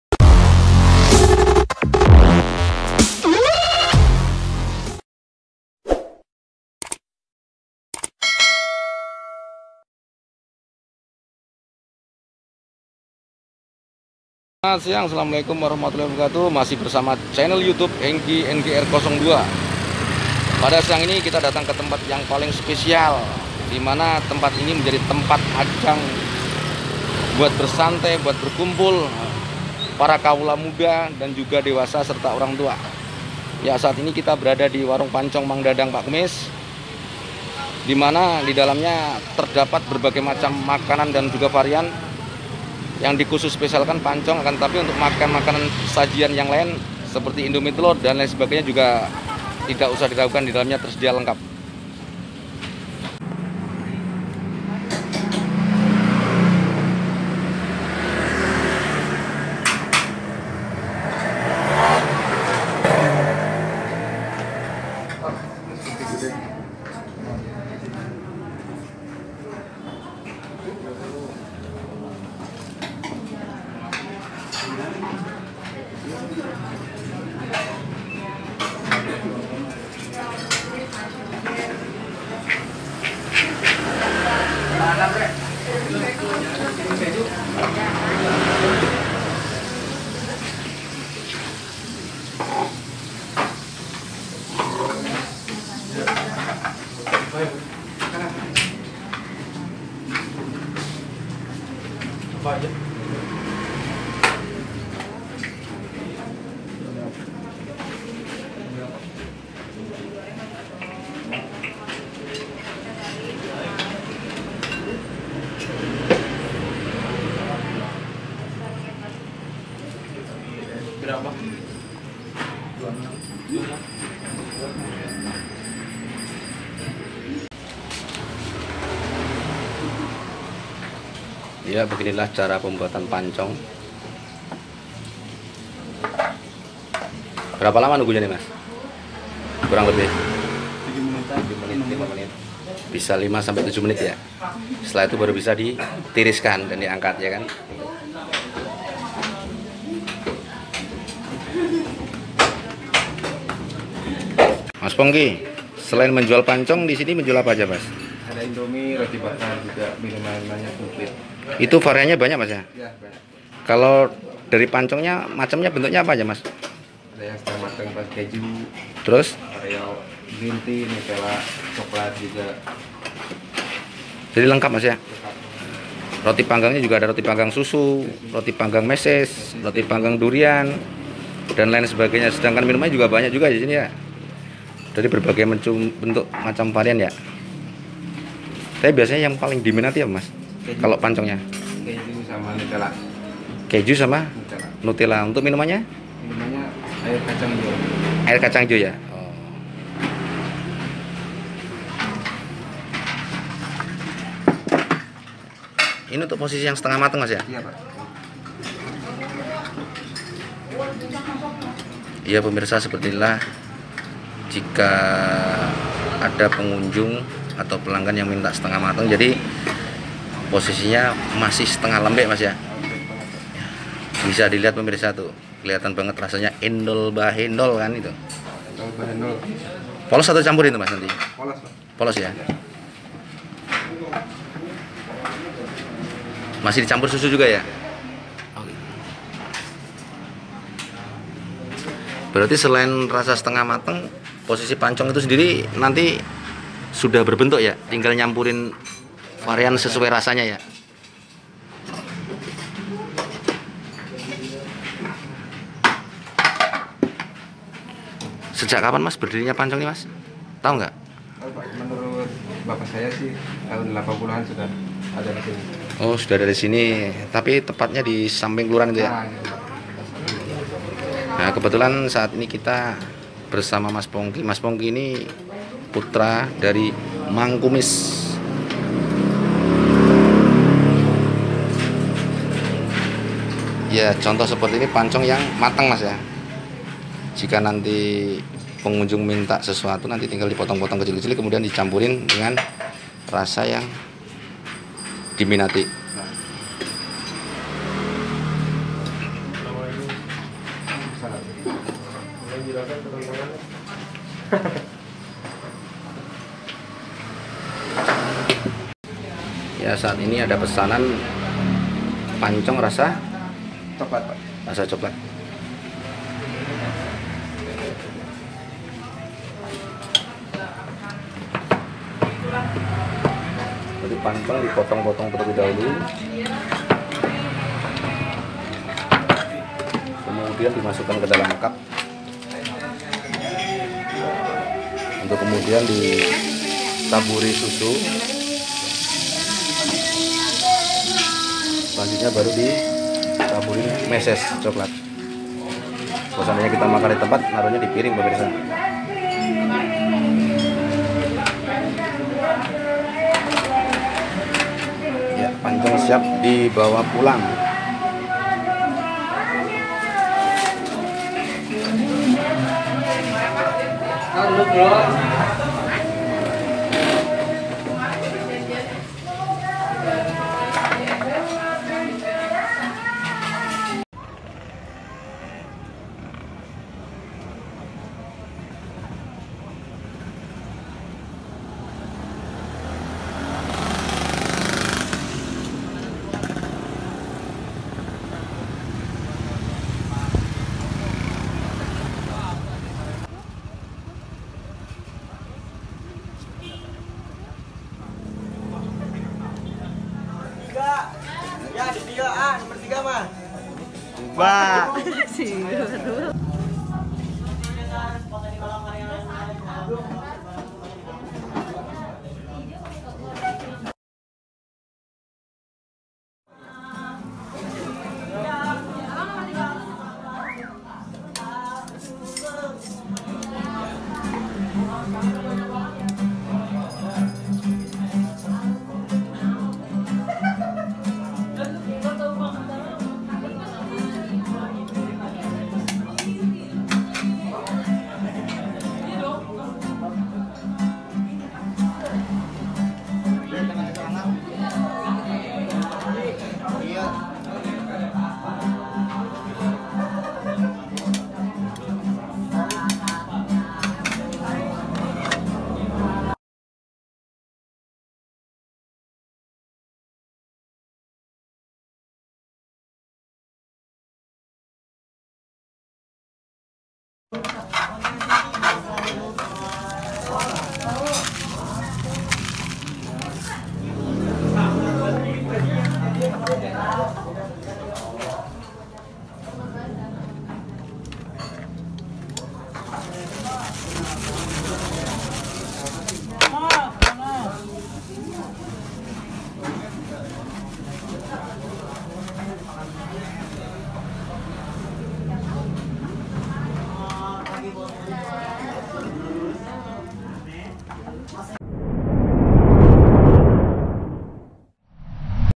Heboh....!!! Ternyata Kue Pancong Yang Paling Mantul Ada Di Depok
Kue Pancong Mang Kumis & H.Dadang Yang Berlokasi Di Jln.Komodo Samping Kelurahan Depok Jaya Kec.Panmas Kota Depok Adalah Legenda Kue Pancong Yang Di Rintis Sejak Tahun 80 an Di Mana Untuk Pembelinya Tidak Hanya Dari Kota Depok..Akan Tetapi Banyak Juga Yang Dari Jakarta, Bandung, Bekasi Dan Kota Besar Lainya.Jika Kalian Penasaran Silahkan Dekati...Rasakan Dan Nikmati Di Warung Pancong Mang Kumis & H.Dadang ( 1x24 Jam ).Di Jamin Harganya Bersahabat Dengan Kantong Dan Jika Mals Jangan Bisa Pesen Melalui Gofood Dll